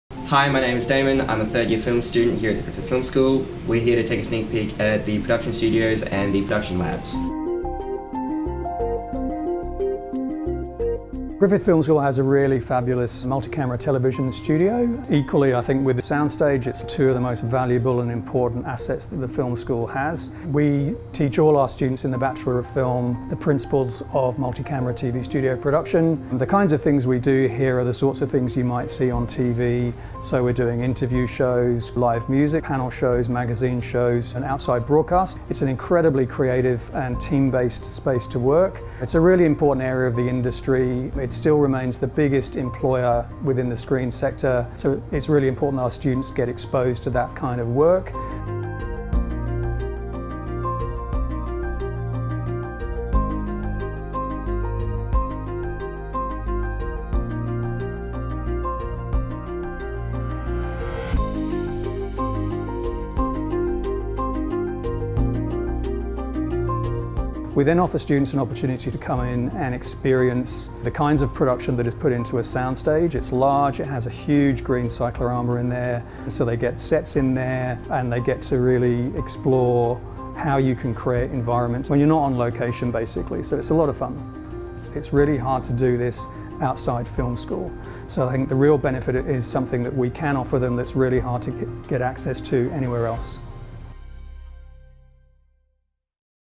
Open House Tour: Griffith Film School
Damon’s in the third year of his degree at Griffith Film School and for spotlight on creative arts and design during Griffith’s Virtual Open House, he takes us for a look around the University’s production studios at the South Bank campus. As Richard Fabb, senior lecturer at Griffith Film School explains, the multi-camera television studio and sound stage offer film students at Griffith an opportunity to use facilities otherwise hard to access. For more information about studying creative arts at Griffith, head to griffith.edu.au/open-house for on-demand presentations, webinars, live chat, downloadables and more.